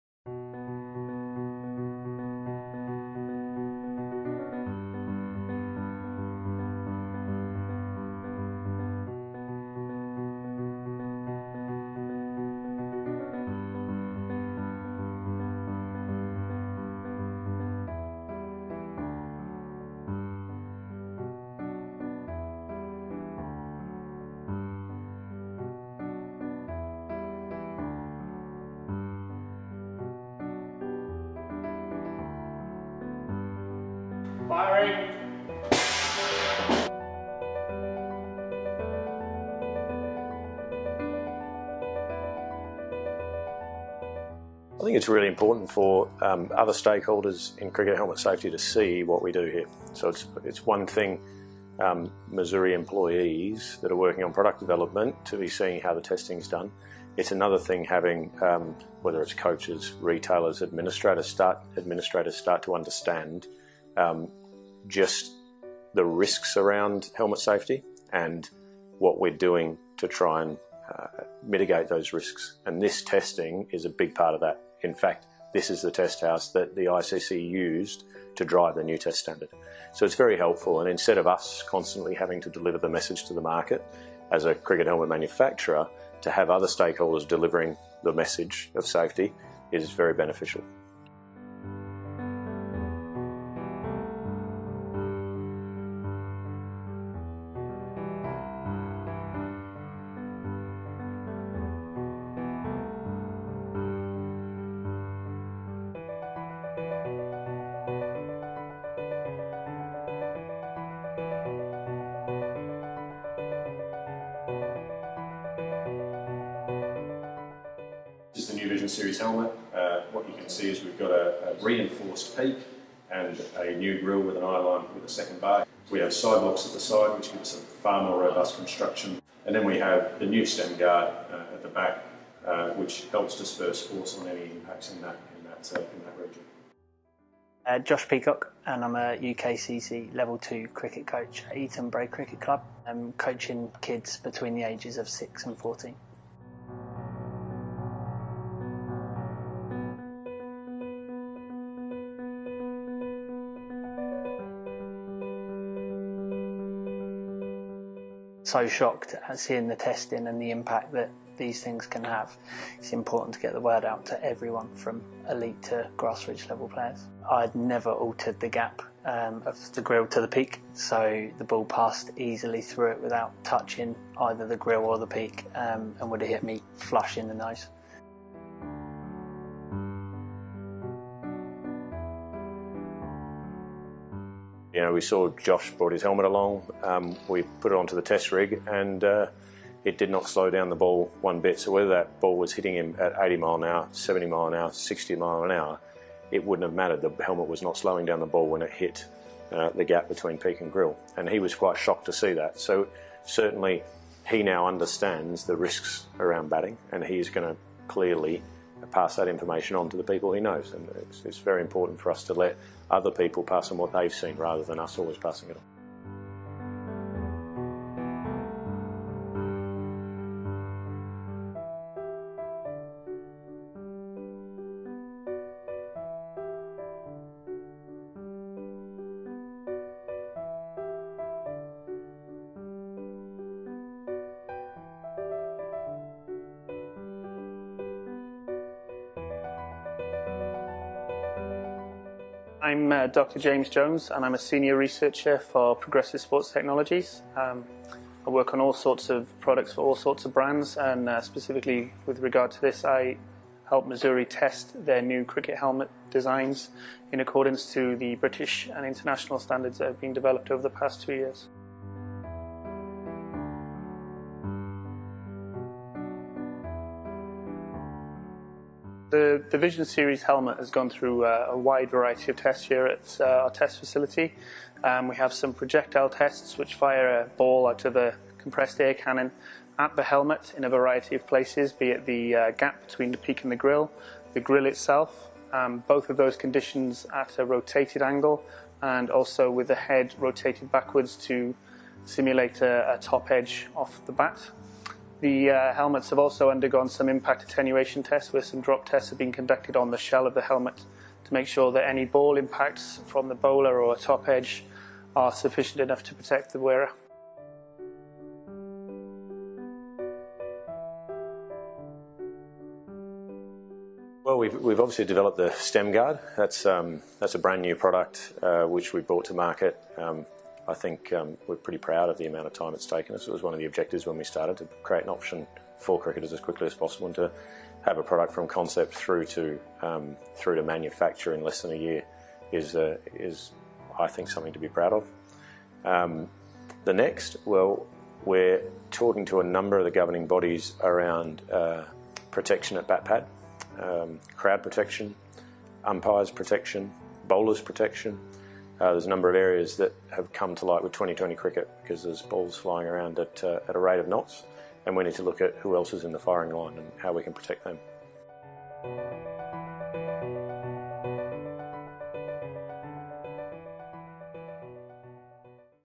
Progressive Sports Technologies' lab | Vision Series helmet testing
Masuri invited ITV News and All Out Cricket magazine to Progressive Sports Technologies' lab to watch the rigorous testing process that Vision Series cricket helmets are subjected to.